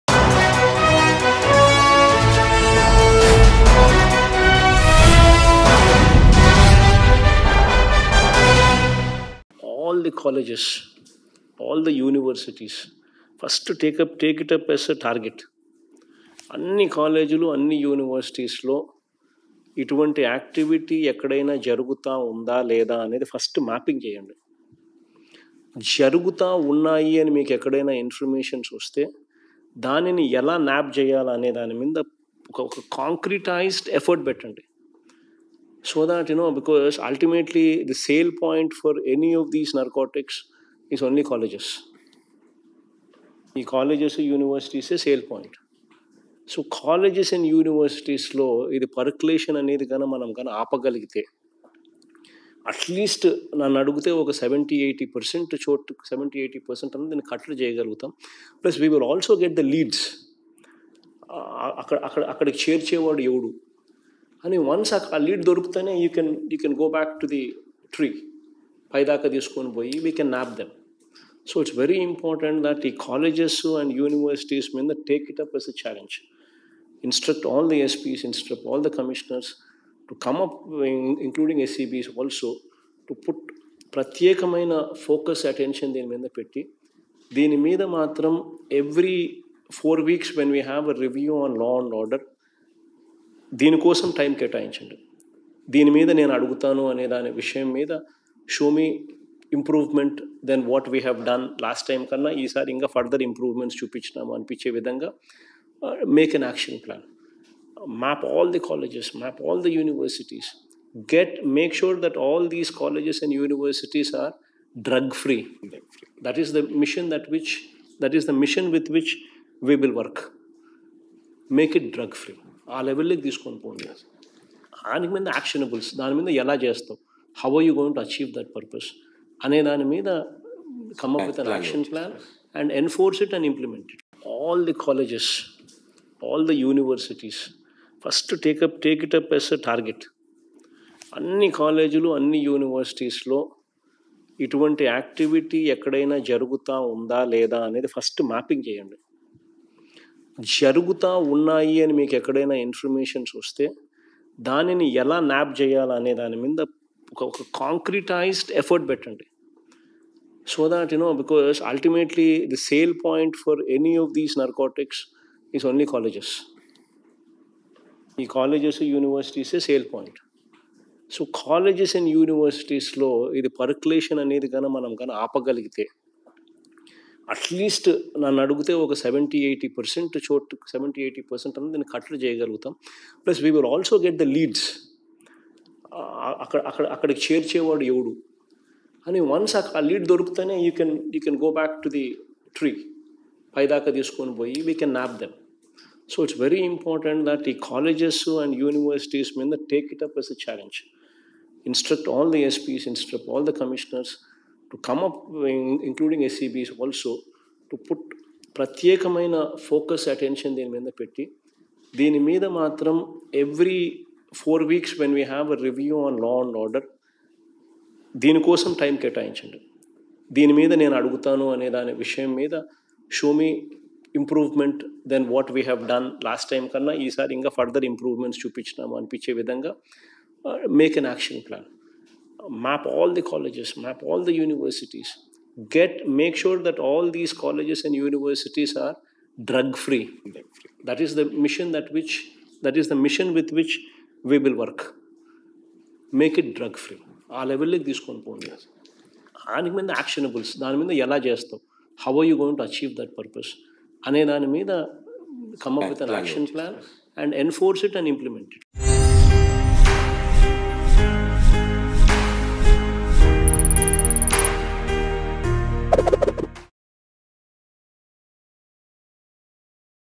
CM YS Jagan Review Meeting On Narcotic Control In AP | AP Govt | AP Latest News | Mango News
CM YS Jagan Review Meeting On Narcotic Control In AP.